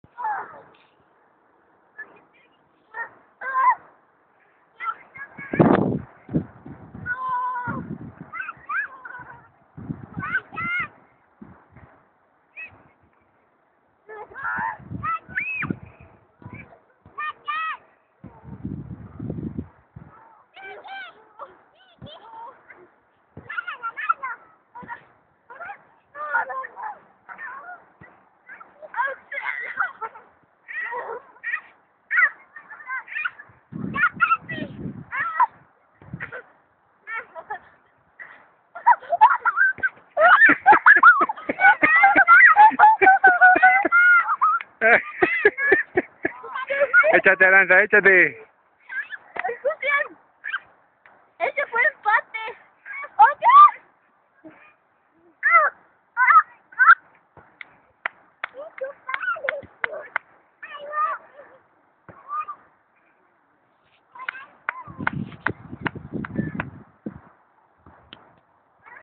Rodando LI y JA. Aranza los sigue.
En el parque de San Gil en San juan del rio, LI y JA en una lucha terminan rodando ambos por la colina que forma el contorno de las canchas de tenis, mientras aranza los observa y se lanza tambien.